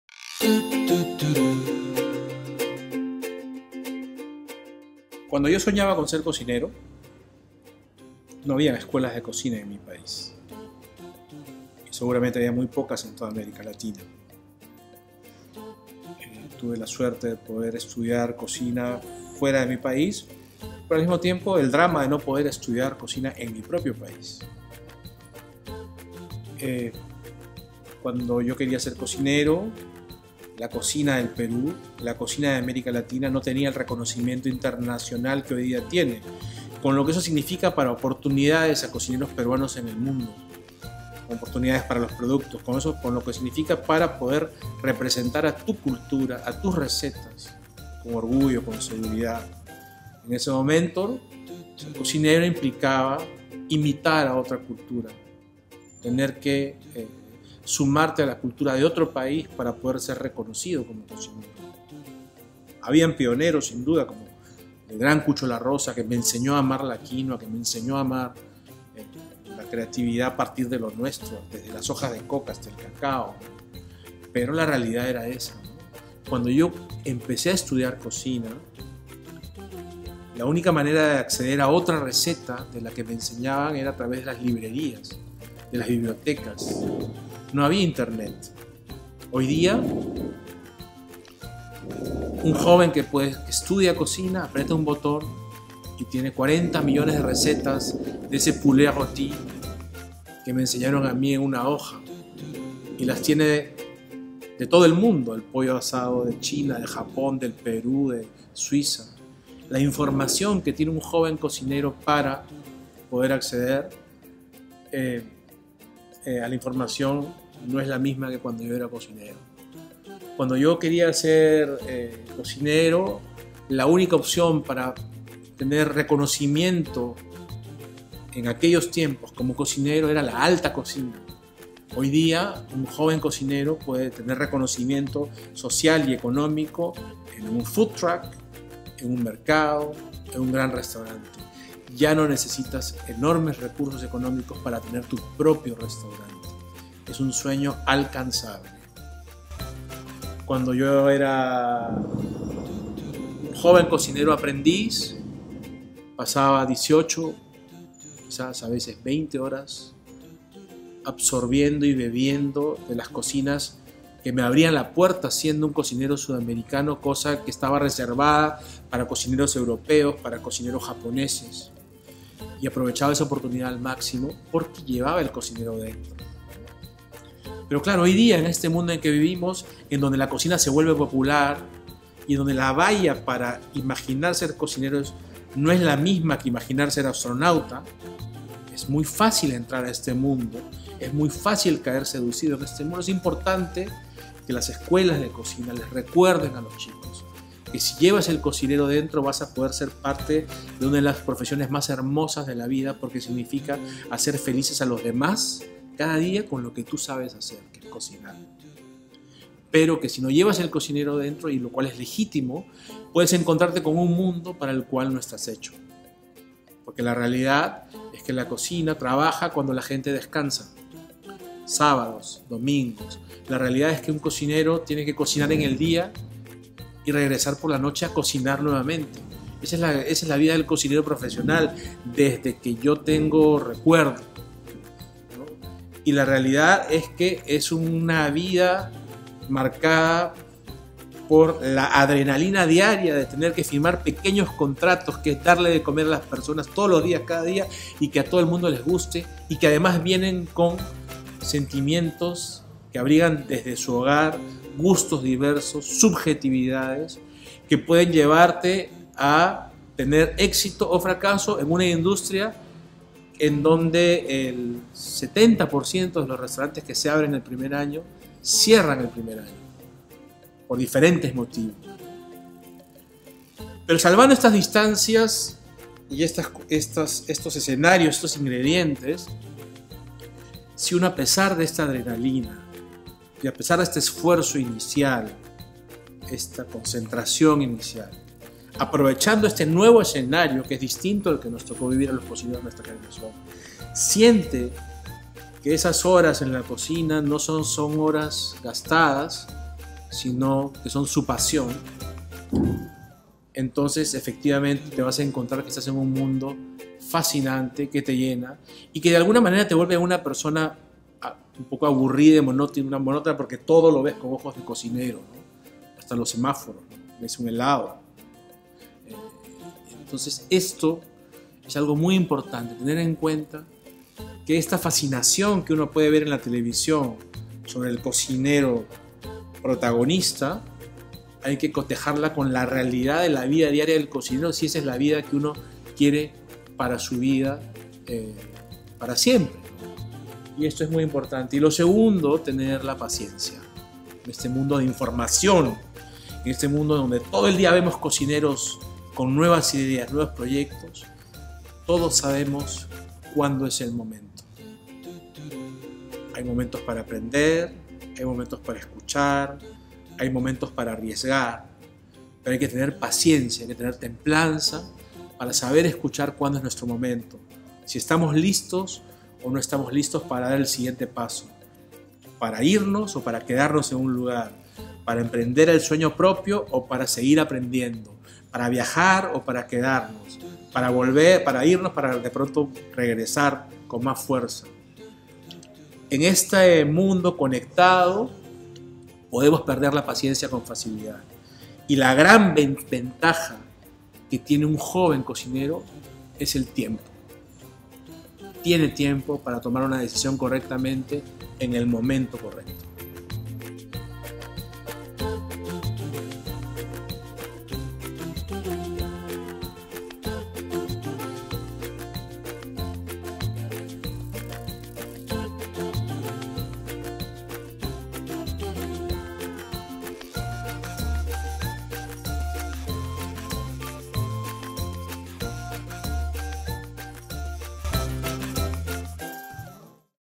Gastón Acurio: ¿Qué aconsejarías a un estudiante que aspira ser un gran cocinero? Sub ingles-español
¿Qué aconsejarías a un estudiante que aspira ser un gran cocinero?

Es algo muy importante tener en cuenta que esta fascinación que uno puede ver en la televisión sobre el cocinero protagonista, hay que cotejarla con la realidad de la vida diaria del cocinero si esa es la vida que uno quiere para su vida para siempre, no? y esto es muy importante, y lo segundo, tener la paciencia en este mundo de información, en este mundo donde todo el día vemos cocineros con nuevas ideas nuevos proyectos, todos sabemos cuando es el momento. Hay momentos para aprender, hay momentos para escuchar, hay momentos para arriesgar, pero hay que tener paciencia, hay que tener templanza para saber escuchar cuando es nuestro momento, si estamos listos o no estamos listos para dar el siguiente paso.

What advice can you give a student who aspires to be a good chef?

It is very important to take in consideration that this fascination that you can see on tv about the lead chef, you have to compare it with the chef’s daily reality, if that is he life that you want for your life forever. This is very important, and in second place, having the patience in this world of information, in this world where we see all day chef with new ideas and projects, we all know when is the moment. There are moments to learn, to listen, to risk, but we have to have patience, temperance to know how to listen when it is our moment, if we are ready or not to take a step forward.